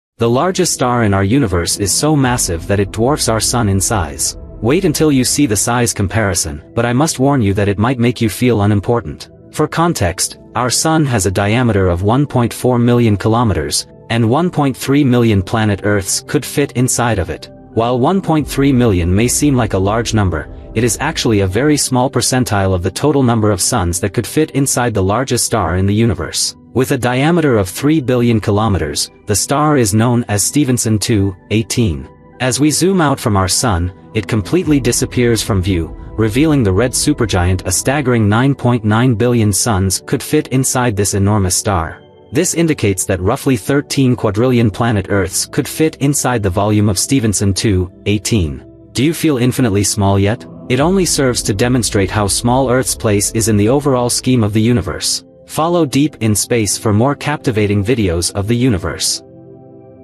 Sun Vs the largest Star, Stephenson 2-18
The largest star in our universe is so massive that it dwarfs our sun in size. Wait until you see the size comparison, but I must warn you that it might make you feel unimportant. For context, our sun has a diameter of 1.4 million km, and 1.3 million planet Earths could fit inside of it. While 1.3 million may seem like a large number, it is actually a very small percentile of the total number of suns that could fit inside the largest star in the universe. With a diameter of 3 billion kilometers, the star is known as Stephenson 2–18. As we zoom out from our sun, it completely disappears from view, revealing the red supergiant a staggering 9.9 billion suns could fit inside this enormous star. This indicates that roughly 13 quadrillion planet Earths could fit inside the volume of Stephenson 2–18. Do you feel infinitely small yet? It only serves to demonstrate how small Earth's place is in the overall scheme of the universe. For more interesting videos of cosmos follow deep in space.